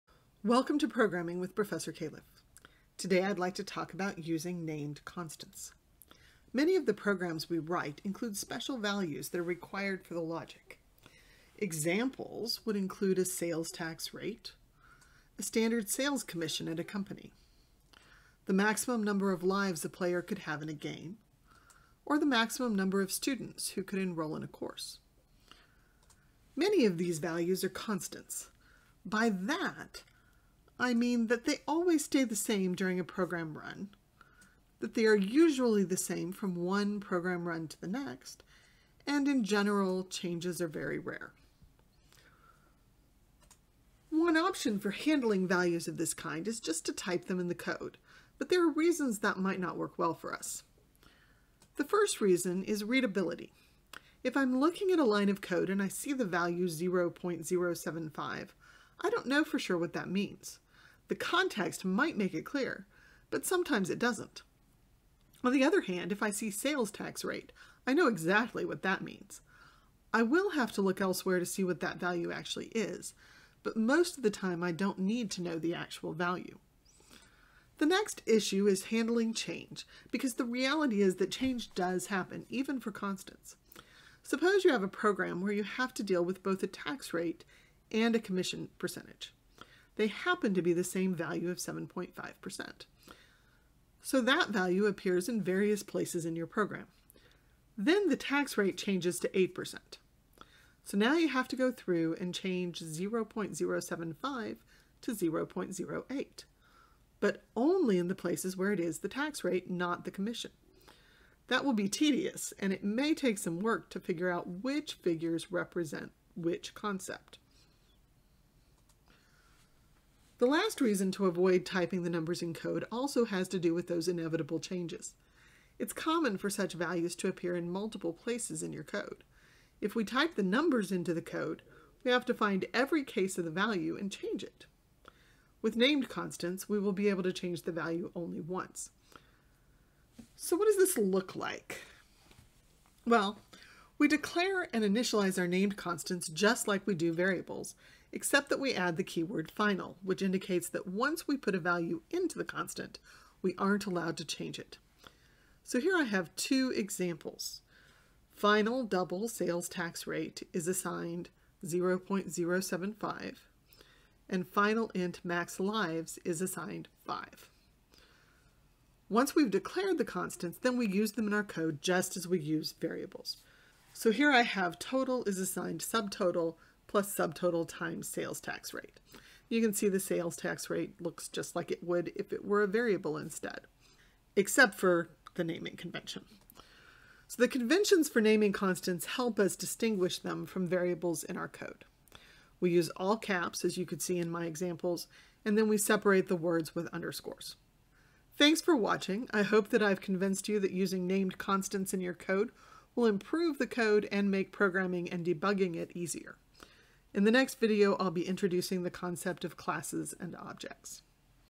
Named Constants in Java
Using named constants in Java, the why and the how.